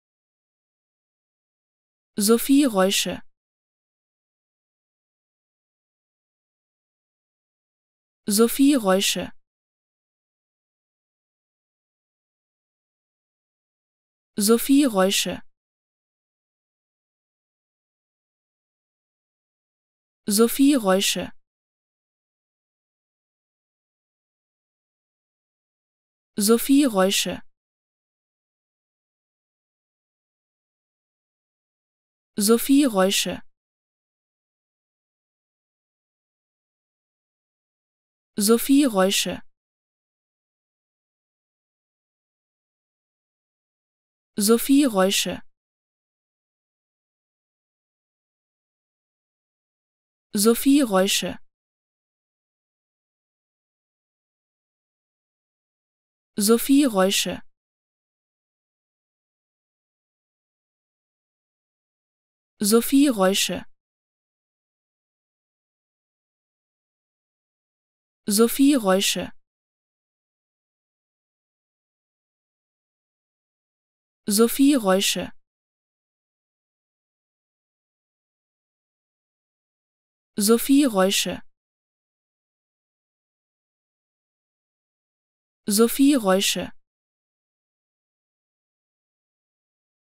How to Pronounce ''Sophie Reusche'' Correctly in German
Learn how to say ''Sophie Reusche'' in German with this short pronunciation tutorial.